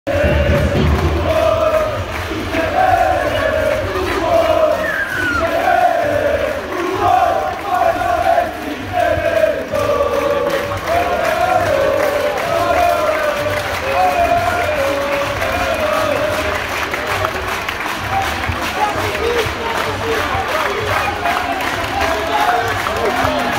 SUNTEM PESTE TOT ACASĂ!🇱🇻🇱🇻🇱🇻
🙏Recunoscători pentru cei mai frumoși suporteri!❤️

Multumim că ne-ați susținut astăzi din tribune, ați fost minunați, vom continua să dăm totul pe teren pentru a vă face mândri de noi!

Forza Rapid!🇱🇻🇱🇻🇱🇻